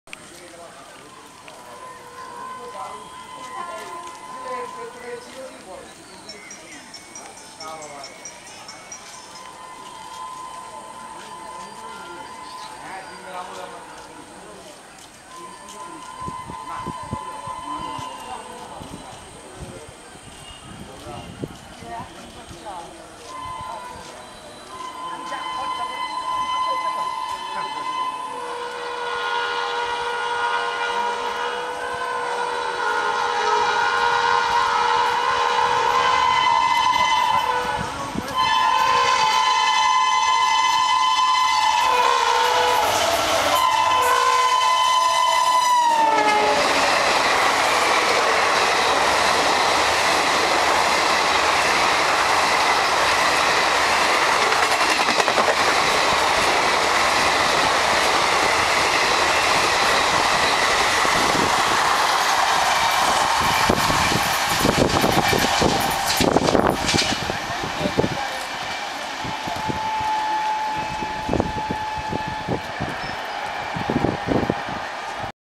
Coalfield Express Passing Through Begampur Station [HD]
12339 UP Howrah - Dhanbad Coalfield Express screamed past Begampur Station on the Howrah - Bardhaman Chord Line Section of ER with a WAP-7.